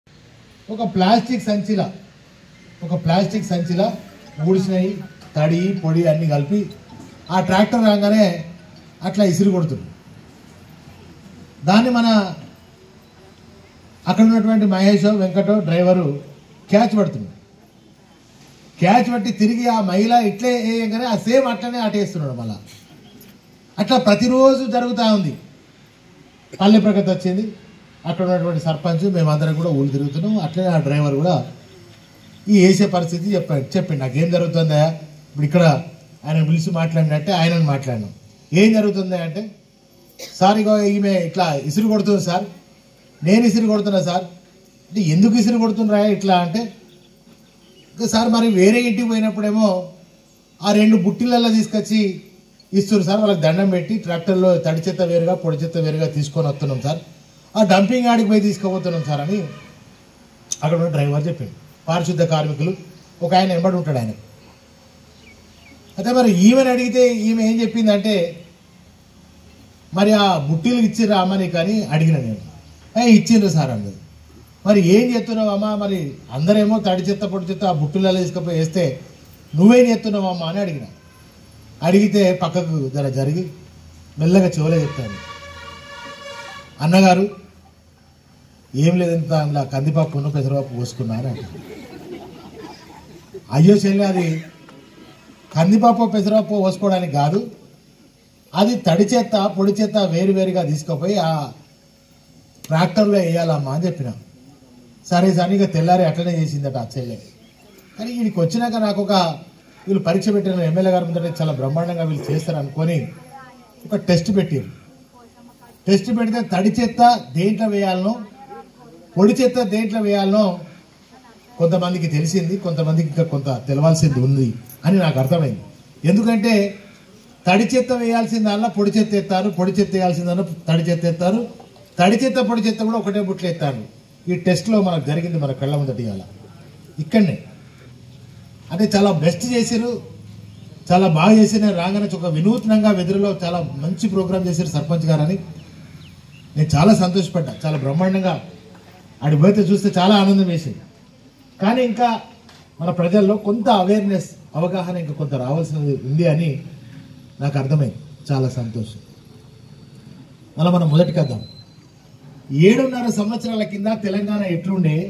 Hon'ble MLA Choppadandi ConstituencynSpeech on Usage of Twin Dust bins and Garbage collection
Hon'ble MLA Choppadandi Constituency speech on Collection garbage daily and usage of Twin dust bins at Palle Pragathi closing day at Vedira of Ramadugu Mandal